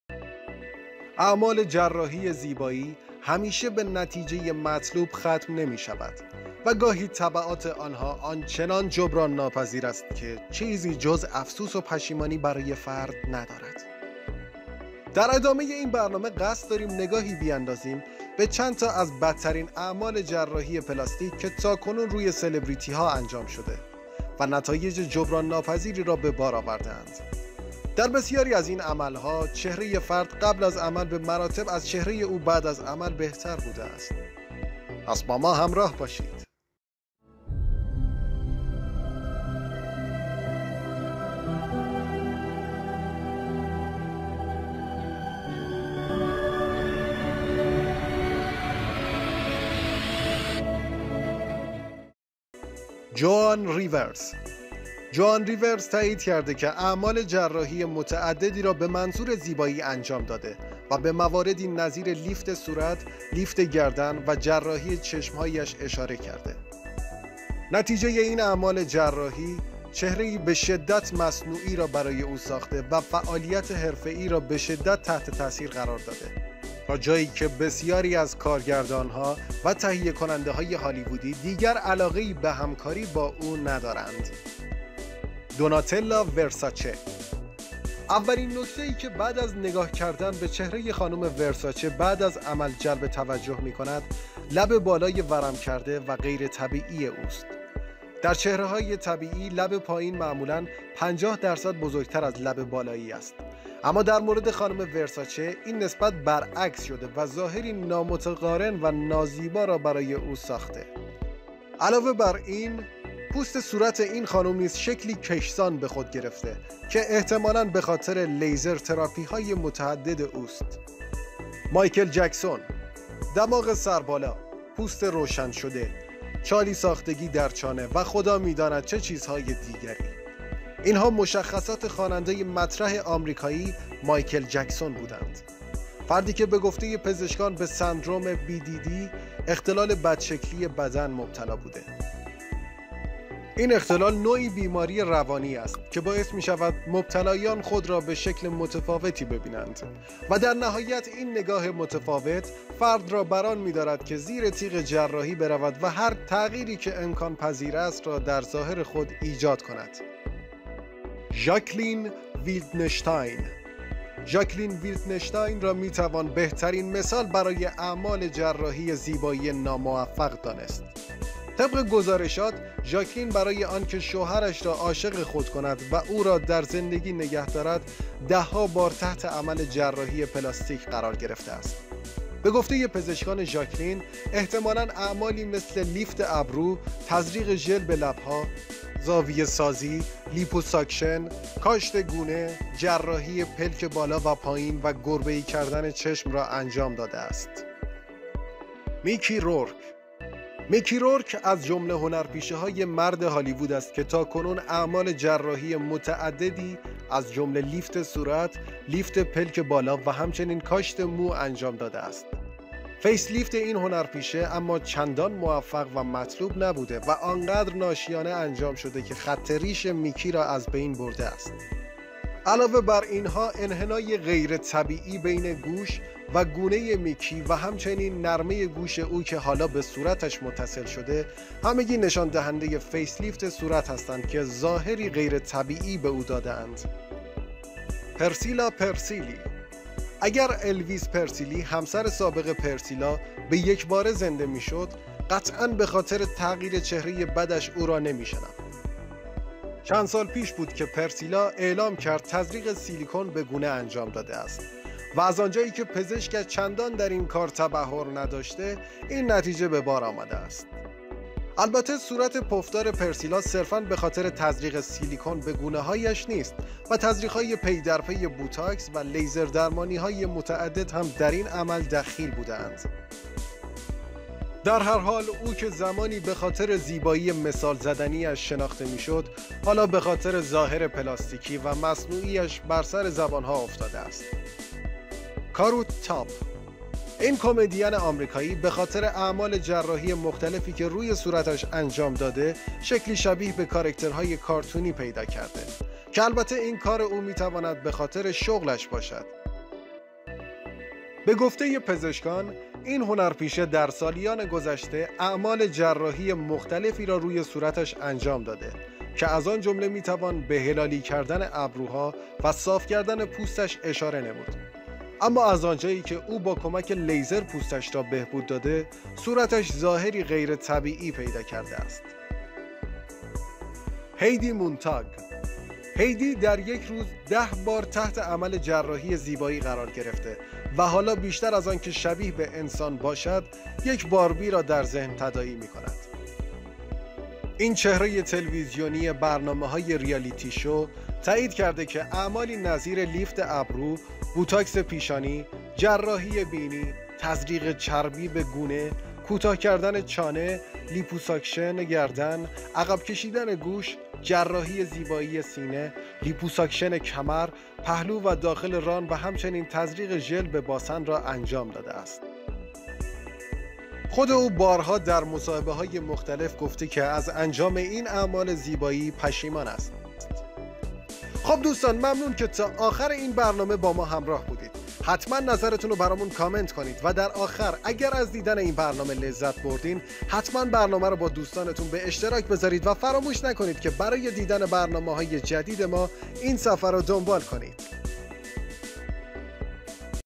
|بدترین عمل های زیبایی جهان | بدترین جراحی زیبایی در دنیا
انجام جراحی زیبایی همواره به نتیجه خوشایند ختم نمی شوند و در برخی موارد تبعات آنها آنچنان ترمیم ناپذیر است که چیزی جز افسوس و ندامت برای فرد ندارند . حال قصد داریم نگاهی بیاندازیم به چند تا از بدترین عمل های جراحی پلاستیک که تاکنون روی سلبریتی ها انجام شده‌ اند و نتیجه های جبران ناپذیری را به بار‌ آورده اند . در زیادی از این عملها چهره فرد قبل از جراحی به مراتب از چهره او بعد از عمل‌ بهتر بوده‌ است .

چند نمونه نتایج ناخوشایند جراحی زیبایی:
0:52 جوان ریورس
1:25 دوناتلا ورساچه
2:01 مایکل جکسون
2:41 ژاکلین ویلدنشتاین
3:18 میکی رورک
پرسیلا پرسیلی 3:58
کاروت تام 4:45
هیدی مونتان 5:23

لطفاً برای حمایت از ما این صفحه را دنبال کنید و با لایک ها و نظرات مثبتتون این صفحه را همراهی کنید